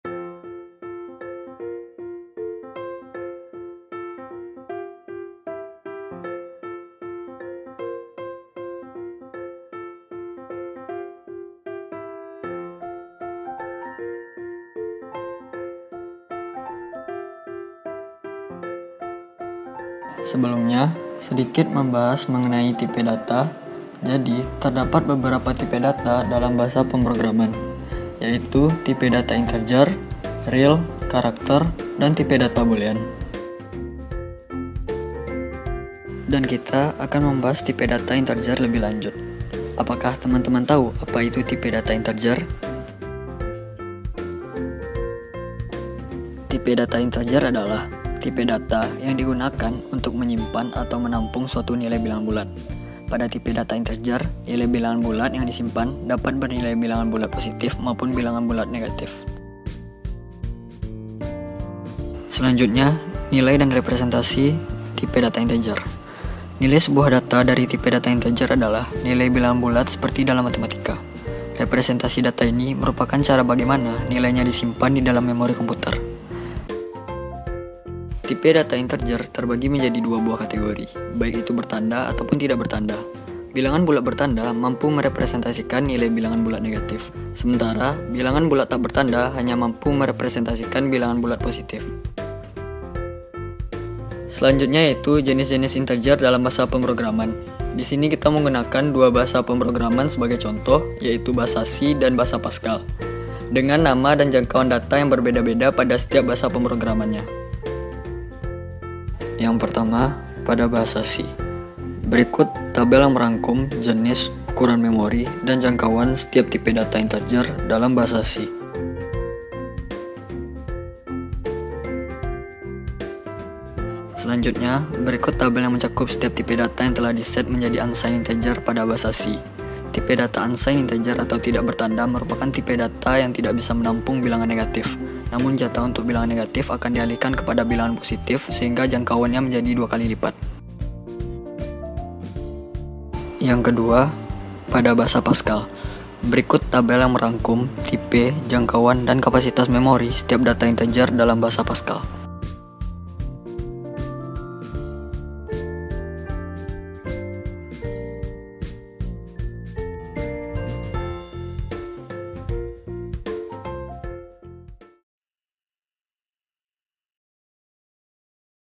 Kelompok 16 || Integer || AOK Kelas A
Tugas Arsitektur dan Organisasi Komputer oleh Kelompok 16 mengenai Integer.
Anggota Kelompok 16:
1. Muhammad Rafvy Octa Nugraha (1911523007)
2. Muhammad Afdhal Syafli (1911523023)